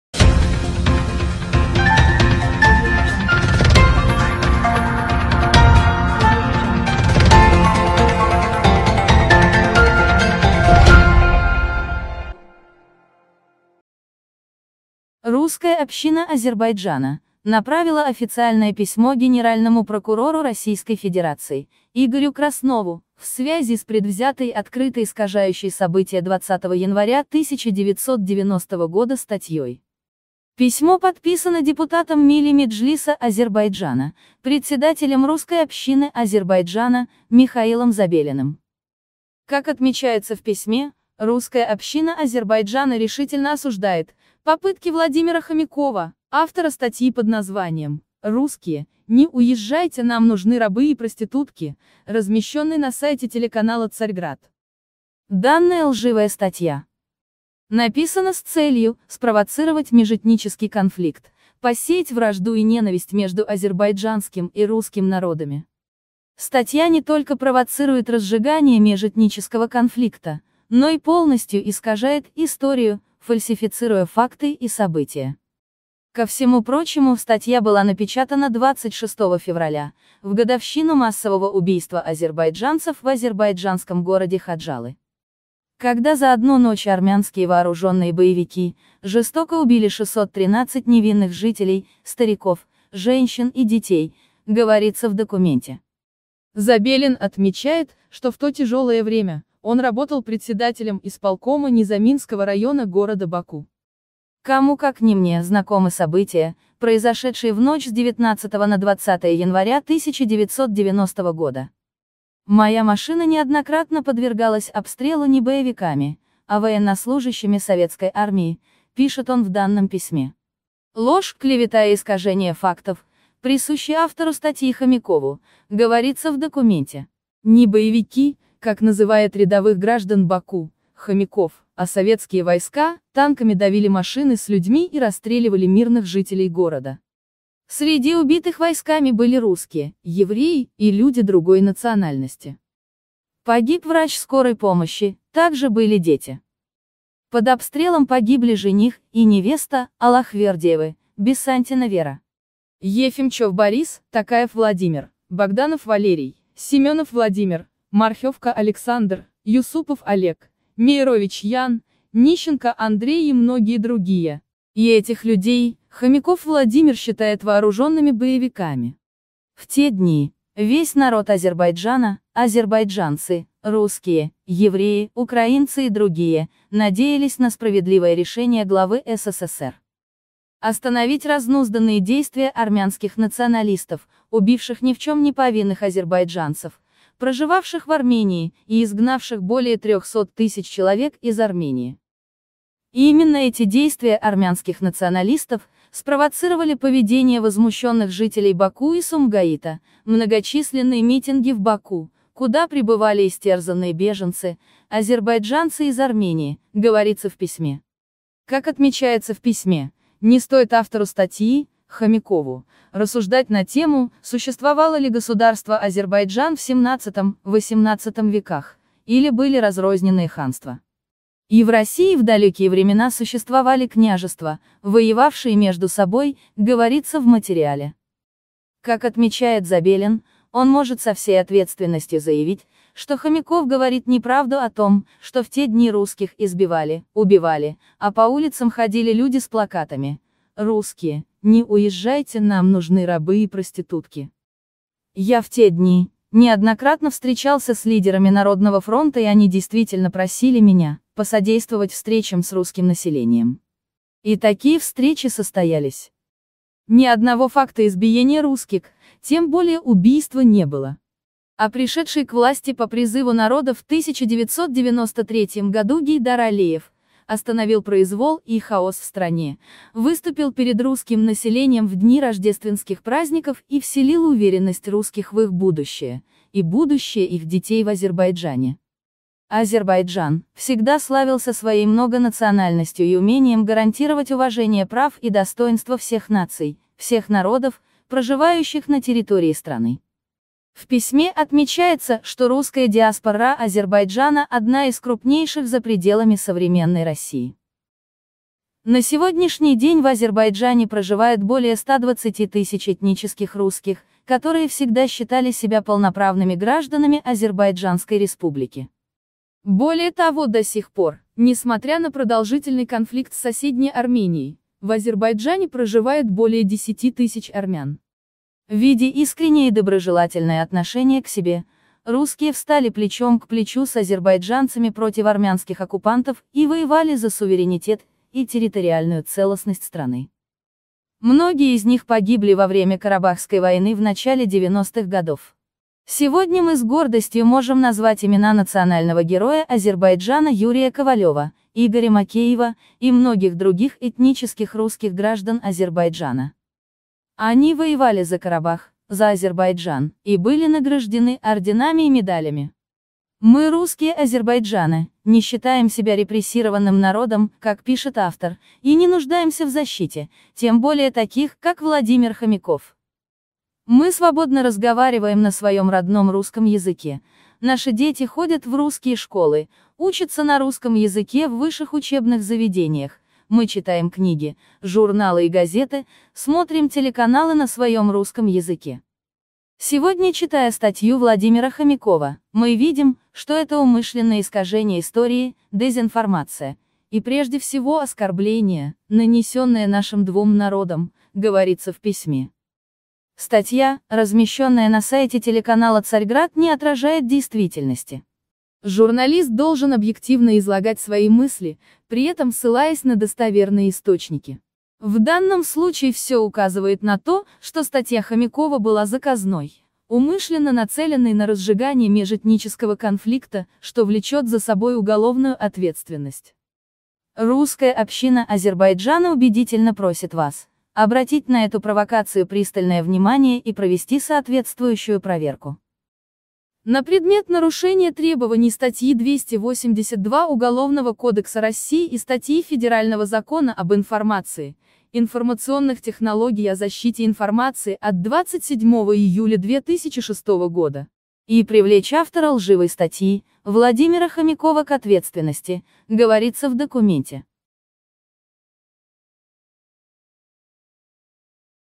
Русская община Азербайджана направила официальное письмо генпрокурору России
Русская община Азербайджана направила официальное письмо генеральному прокурору Российской Федерации Игорю Краснову в связи с предвзятой и открыто искажающей события 20 января 1990 года, статьей
Владимира Хомякова, размещённой на сайте телеканала "Царьград"